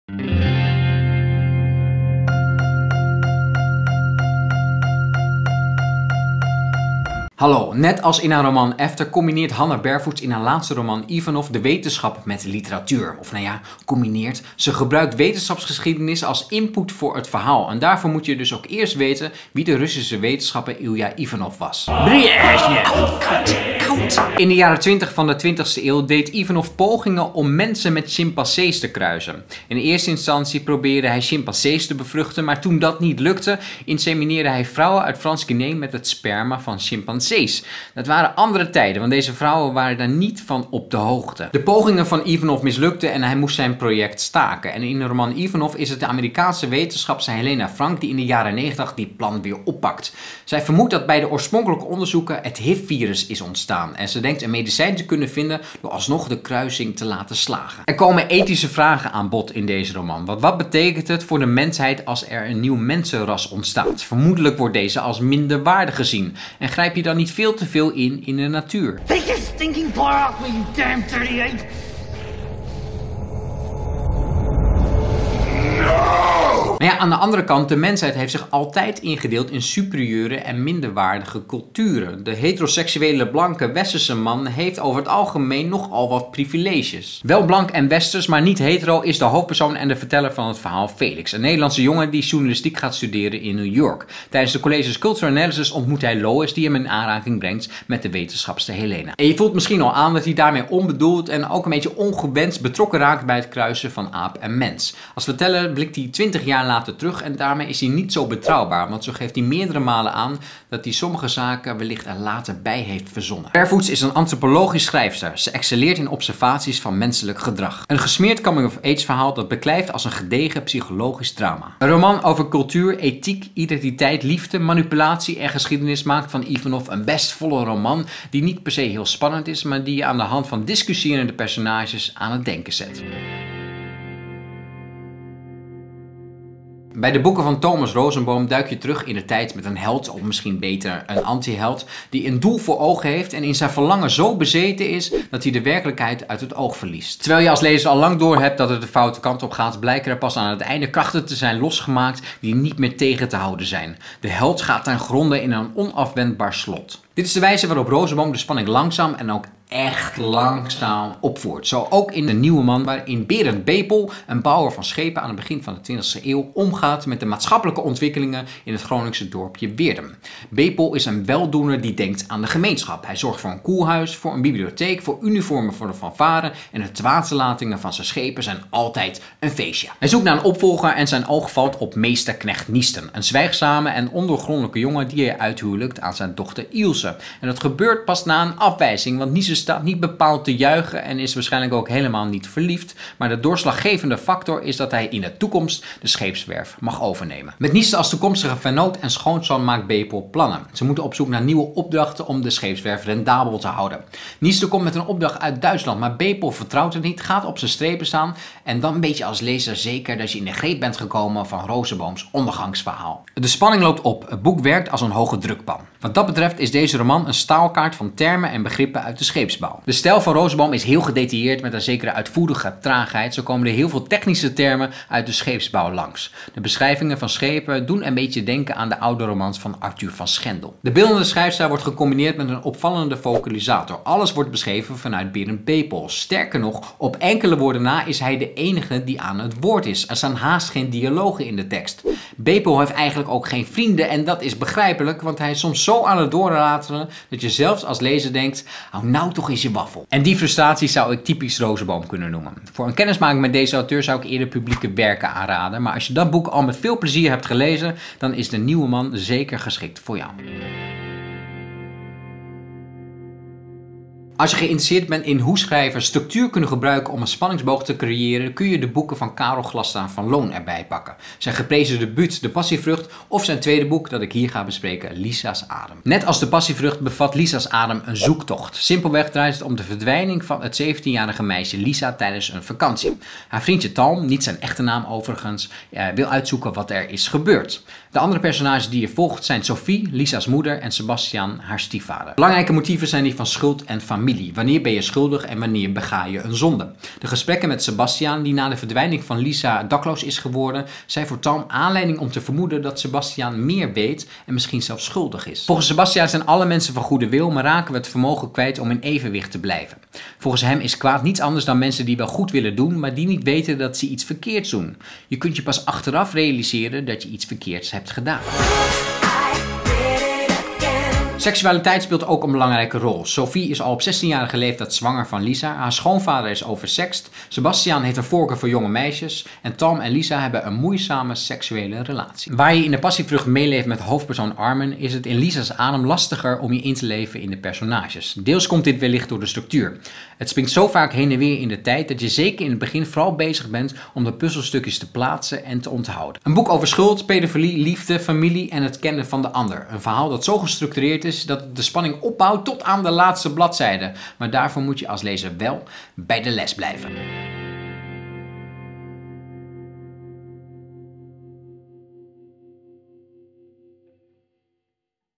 Vlogboek84 - Hanna Bervoets / Thomas Rosenboom / Karel Glastra van Loon
In deze video bespreekt Jörgen de volgende drie boeken:
Hanna Bervoets - Ivanov
Thomas Rosenboom - De nieuwe man
Karel Glastra van Loon - Lisa's adem

Vlogboek bespreekt in elke video heel kort drie Nederlandse romans. Binnen enkele minuten heb je een goed beeld van deze boeken en weet je of ze je wel of niet aanspreken.